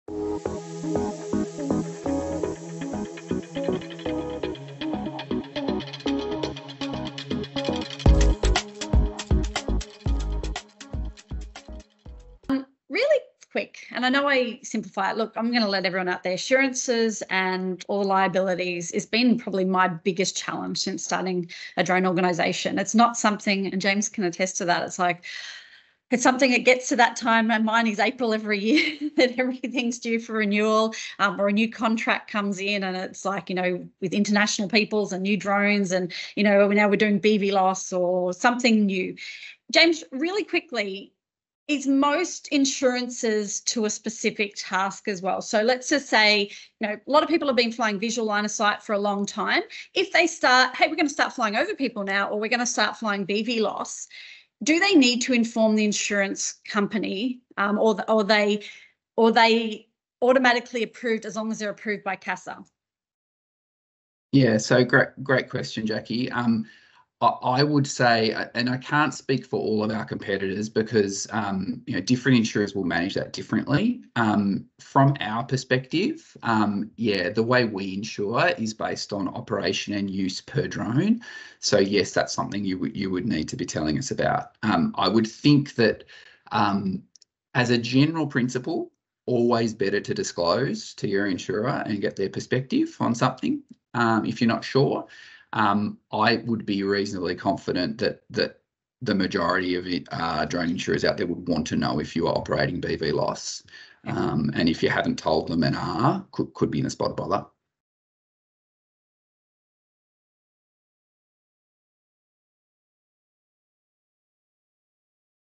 Drone Insurance - Disclosure
Food for Thought: Should You Notify Your Insurer About New Drone Operations?
James Ainslie from Precision Autonomy recently answered an important point worth reflecting on. Should you notify your insurer if you plan to launch new operations for the first time or expand into areas like Beyond Visual Line of Sight (BVLOS) or Operations Over or Near People (OONP)?
The short answer: Yes, disclosing such changes is always wise. Proactively communicating with your insurer helps ensure your coverage aligns with the evolving risks of your operations. New activities bring unique challenges; without appropriate insurance adjustments, you could be exposed to unexpected liabilities.
It’s always better to be proactive. Confirming with your insurer now can save you from potential complications down the track.